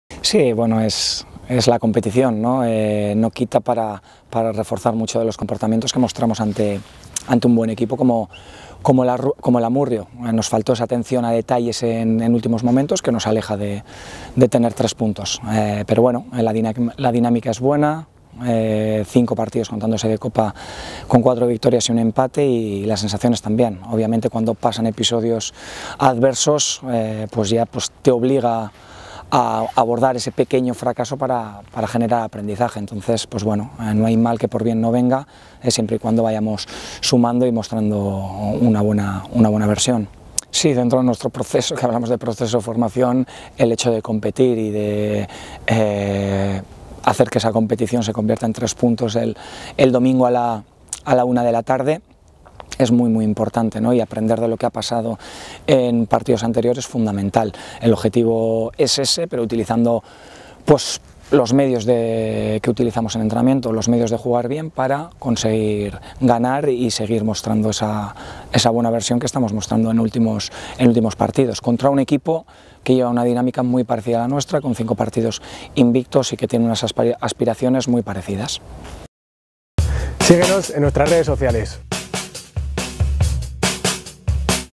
PREVIA ALAVÉS B - ZAMUDIO
El entrenador albiazul Igor Oca analiza en el encuentro del filial alavesista frente al Zamudio.